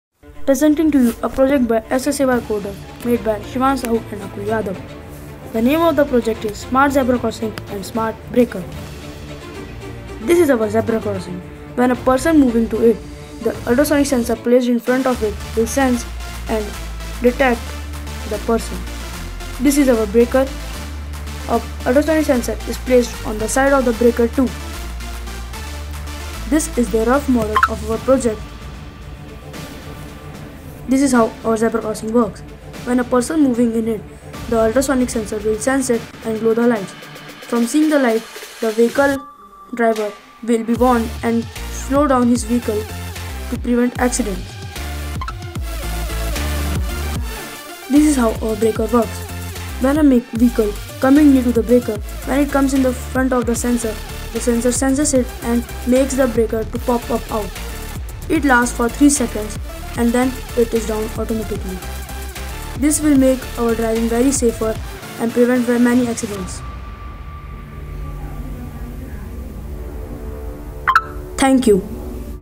Smart Zebra Crossing and Breaker
This is a hardware based project,pls watch and visualise it
A Video by SSAY COADERS
Shivansh Sahu
Akul Yadav
Delhi Public School,Bhilai